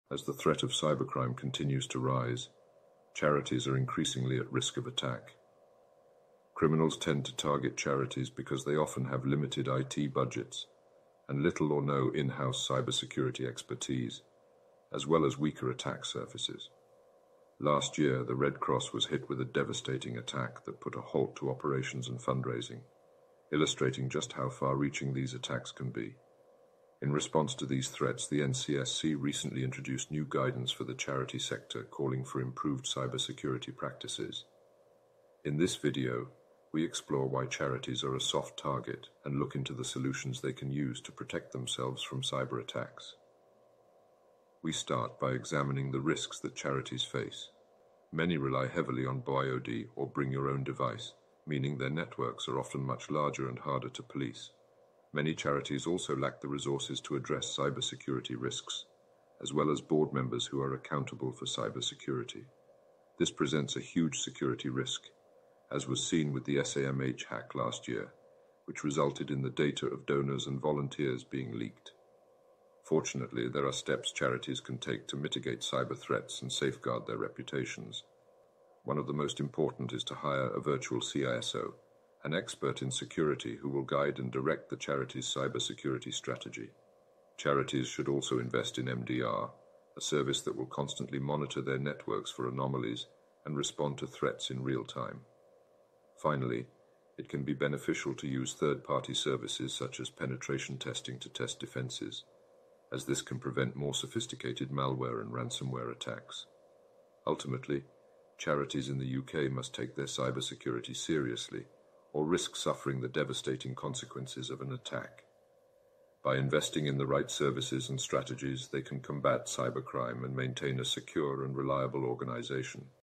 Charities: Know About Ransomware & Reputation Risks
Charities provide valuable services to those in need and they are an important part of the global infrastructure. Unfortunately, cybercriminals are targeting charities with malicious software and ransomware, putting organizations, donors, volunteers and beneficiaries at risk. In this video, we explain what charities should know about ransomware and reputational threats.

From data breaches to ransomware and leaked confidential information, cyber security is one of the greatest threats facing charities today. We discuss why charities are increasingly targeted by cybercriminals, the dangers associated with ransomware, and how to protect yourself. We also discuss the reputational risk of not properly safeguarding private information and the importance of being proactive in your security steps.

We also cover the following topics:

• What is ransomware?
• Why are charities targeted by cybercriminals?
• The dangers of falling for ransomware.
• How to protect charities from cyberattack.
• The dangers of a data breach.
• Reputational risks: What happens when private information is leaked.
• Steps for protecting confidential data.

By the end of this video, charities will be better informed on the risks associated with ransomware and data breaches, and more aware of the steps they can take to protect themselves.